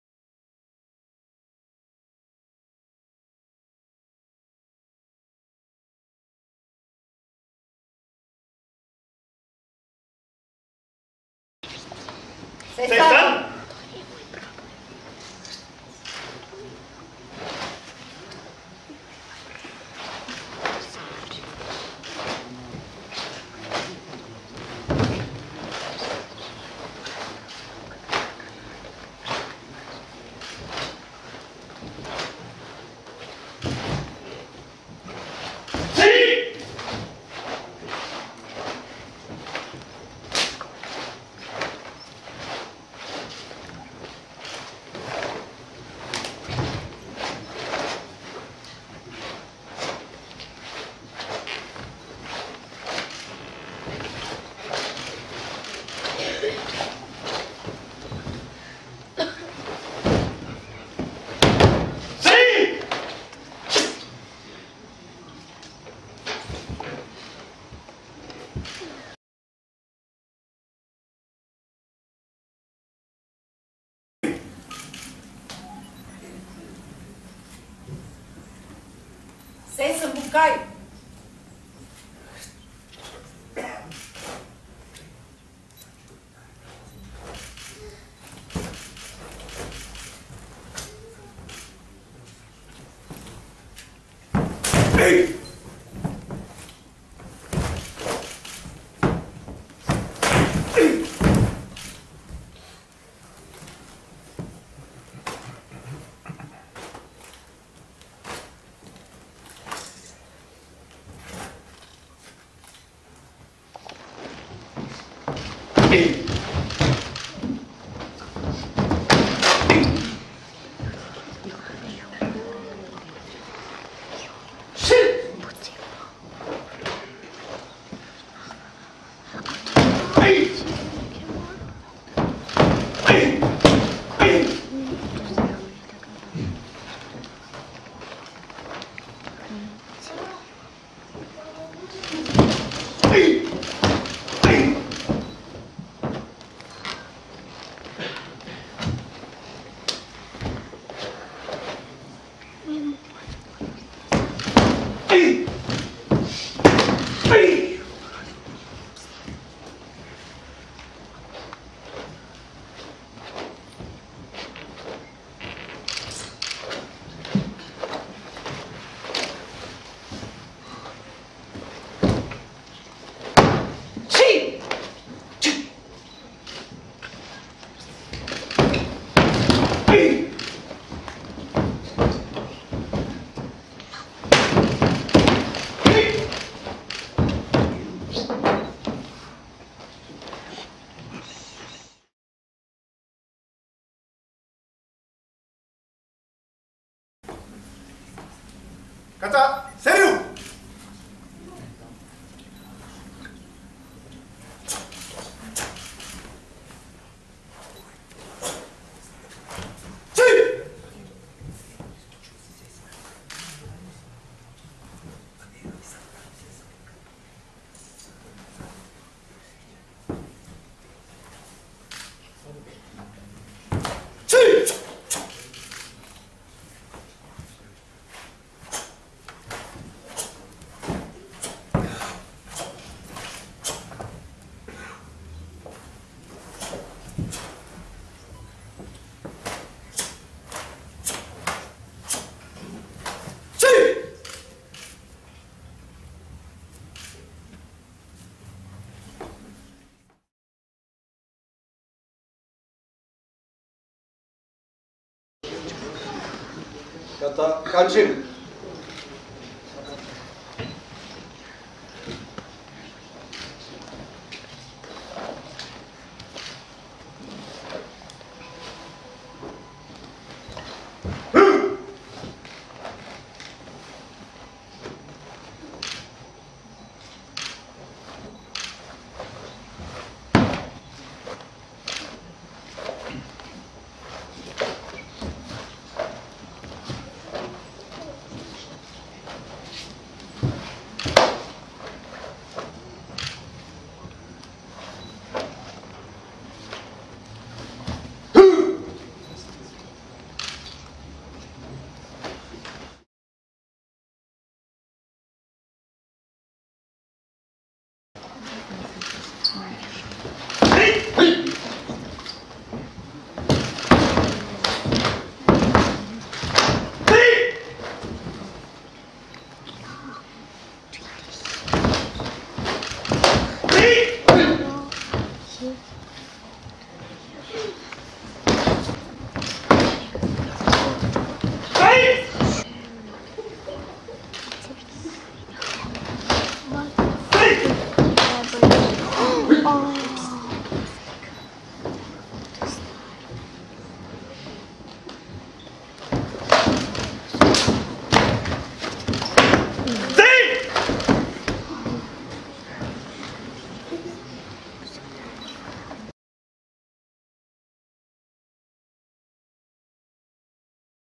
沖縄空手道 SERBIA UECHI-RYU 40th ANNIVERSARY ENBUKAI (2 of 3)
The 40th Anniversary of the Uechi-ryu Okinawa Karate-Do in Serbia [Enbukai], Belgrade, 10th November 2013. Seisan Dantai Kata (Tatjana Zakic, Milos Panajotov & Stefan Jelic, Serbia), Seisan-Bunkai (Milan Dimitrieski, Tatjana Zakic & Vladimir Djordjevic, Serbia), Seiryu-Kata (Daniel Bjelivuk, Croatia), Kanchin-Kata (Rade Samardzija, Serbia), Yakusoku-Kumite (Vladimir Djordjevic & Milos Panajotov, Serbia) --- セルビア上地流沖縄空手道40周年、ベオグラード市、2013年11月10日。 演武会。 十三（セーサン） : ザキッチ・タティアナ、パナヨトヴ・ミロシゥ と ヤェリッチ・ステハン [セルビア]。 十三（セーサン）分解: ディミトレスキ・ミラン、ザキッチ・タティアナ と ジォルジェヴィッチ・ヴラディミル [セルビア]。 十六（セーリュウ）: ビヤェリヴク・ダニエル [クロアチア]。 完戦（カンチン）: サマルジヤ・ラーデ [セルビア]。 約束組手 : ジォルジェヴィッチ・ヴラディミル と パナヨトヴ・ミロシゥ [セルビア]。